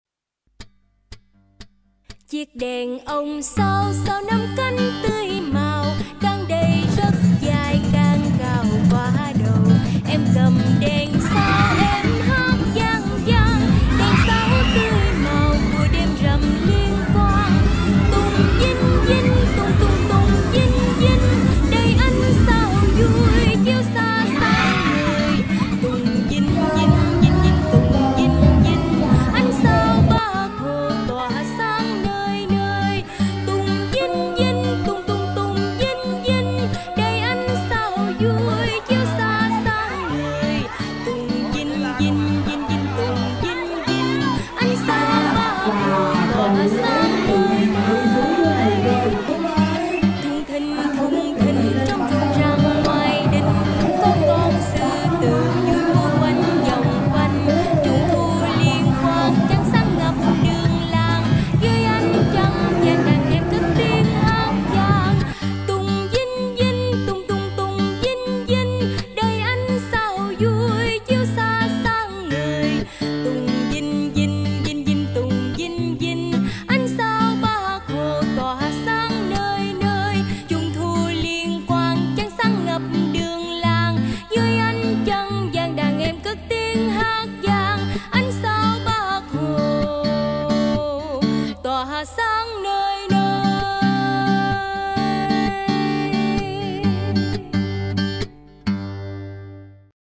Vui Tết Trung Thu 2023 tại Làng Chăm Vĩnh Hanh (Châu Thành, An Giang)
Nhóm LTAG đã thay mặt Mạnh Thường Quân gửi 420 phần quà trung thu và phối hợp với Đoàn thanh niên xã Vĩnh Thành tổ chức vui trung thu cho trẻ em tại Thánh Đường Jamiul Mukminin (Vĩnh Hanh, Châu Thành).

© Nhóm Lang Thang An Giang